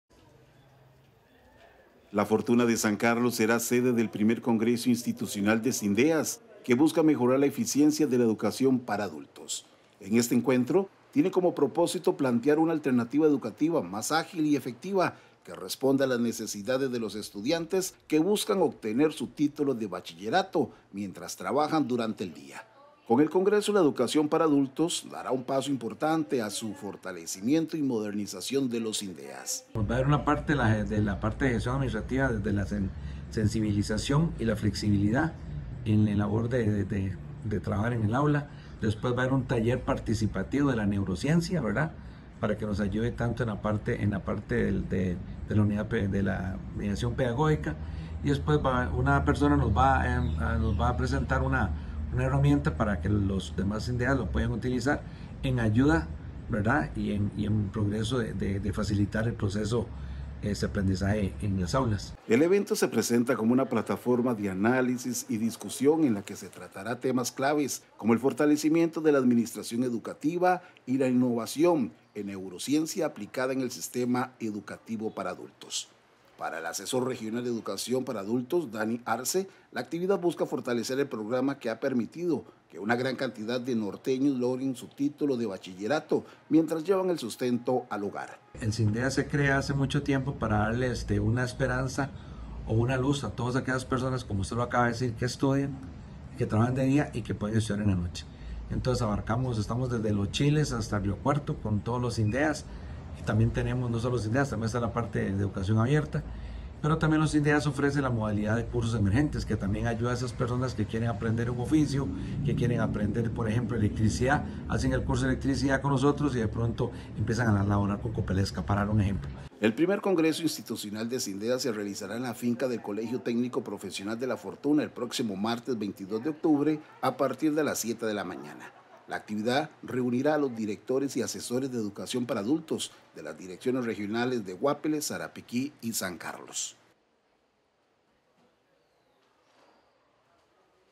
LA FORTUNA SERÁ SEDE DEL CONGRESO INSTITUCIONAL DE CINDEAS
En esta jornada participaran las direcciones regionales de educación de Guápiles, Sarapiquí y San Carlos.